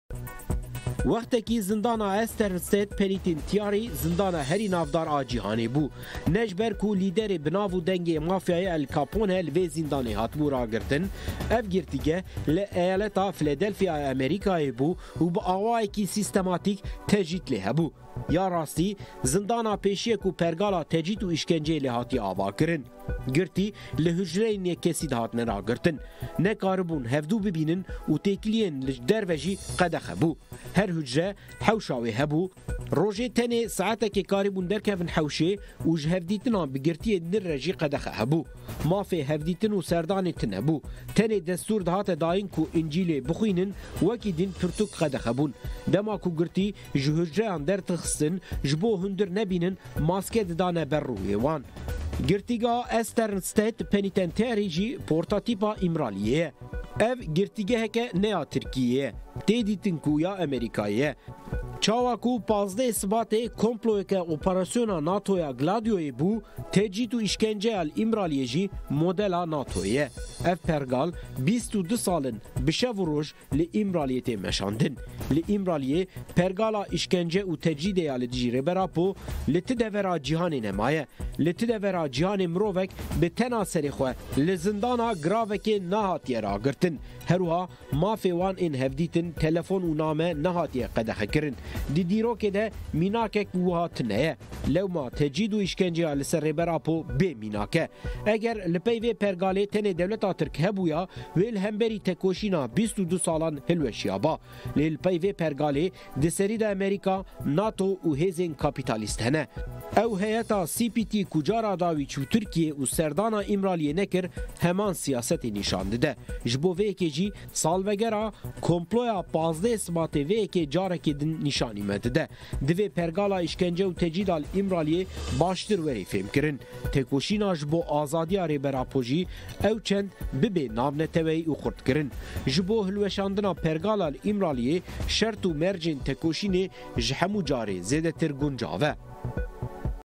Meral Çîçek: Zindaneke NATO'yê Îmralî 10.02.2021
ZINDANEKE NATO'YÊ ÎMRALI
MERAL ÇÎÇEK / YENÎ OZGUR POLÎTÎKA

Wextekî zindana Eastern State Penitentiary zindana herî navdar a cîhanê bû. Lê ne ji ber ku lîderê bi nav û deng ê mafyayê Al Capone li vê zindanê hatibû ragirtin. Ev girtîgeh li eyaleta Philadelphia ya Amerîkayê bû û bi awayekî sîstematîk tecrîd lê hebû. Ya rastî zindana pêşiye ku pergala tecrît û êşkenceyê lê hatiye avakirin.

Girtî li hucreyên yek kesî dihatin ragirtin. Ne karîbûn hevdû bibînin û têkiliya bi derve re jî qedexe bû. Her hucre hewşa wê hebû. Rojê tenê saetekê karîbûn derkevin hewşê û ew jî hevdîtina bi girtiyên din re qedexe bû. Mafê hevdîtin û serdanê tinebû. Tenê destûr dihat dayîn ku Încîl bixwînin, wekî din pirtûk qedexe bûn. Dema ku girtî ji hucreyan derdixistin ji bo hundir nebînin maske didan ber rûyê wan.

Girtîgeha Eastern State Penitentiary jî prototîpa Îmraliyê ye. Ev girtîgeheke ne ya Tirkiyê ye. Tê dîtin ku ya Amerîkayê ye. Çawa ku 15'ê Sibatê komployeke operasyona NATO-Gladyo bû, tecrît û êşkenceya li Îmraliyê jî modela NATO ye. Ev pergal 22 sal in bi şev û roj li Îmraliyê tê meşandin.

Li Îmraliyê pergala êşkence û tecrîdê ya li dijî Rêber Apo li ti devera cîhanê nemaye. Li ti devera cîhanê mirovek bi tena serê xwe li zindana giravekê nehatiye ragirtin. Her wiha mafê wan ên hevdîtin, telefon û name nehatiye qedexekirin. Di dîrokê de mînakek wê tineye. Lewma tecrît û êşkenceya li ser Rêber Apo bê mînak e.

Eger li pey vê pergalê tenê Dewleta Tirk hebûya wê li beramberî têkoşîna 22 salan hilweşiya ba. Lê li pey vê pergalê di serî de Amerîka, NATO û hêzên kapîtalîst hene. Ew heyeta CPT ku cara dawî çû Tirkiyê û serdana Îmraliyê nekir heman siyasetê nîşan dide.

Ji bo vê yekê jî salvegera komploya 15'ê Sibatê vê yekê careke din nîşanî me dide. Divê pergala êşkence û tecrîdê ya li Îmraliyê baştir were fêmkirin. Têkoşîna ji bo azadiya Rêber Apo jî ew çend bibe navnetewî û xurtkirin. Ji bo hilweşandina pergala li Îmraliyê şert û mercên têkoşînê ji her carê zêdetir guncav e.

Stêrk tv li ser şopa heqîqetê.
Ji bo hûn li kanala me Stêrk tv temaşe bikin, hûn dikarin bibin endam û rûpela me parve bikin.